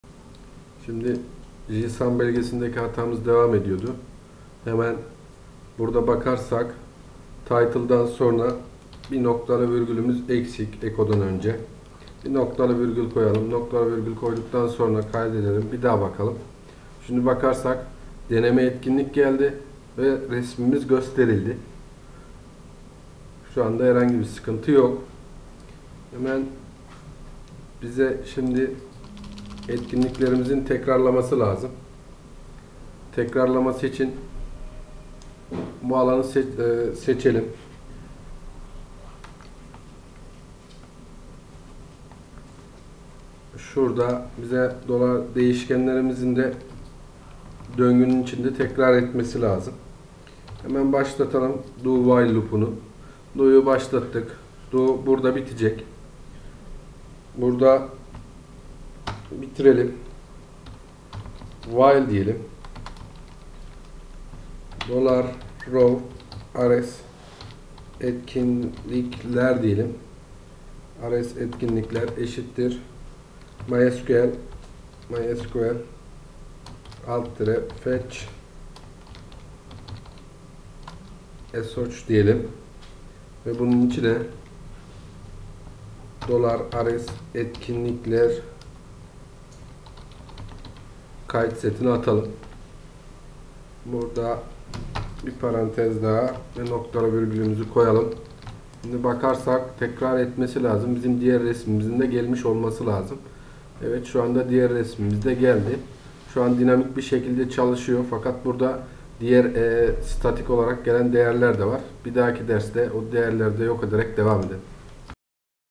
06 Json dosyasının düzenlemesi 2
Bu Eğitimde Jquery Etkinlik Takvimi Uygulamasını PHP & MYSQL ile Dinamik olarak nasıl kullanacağımızı göreceğiz.Eğitim
yaklaşık 2 yıl önce çekildiği için DW CS4 kullanılmıştır.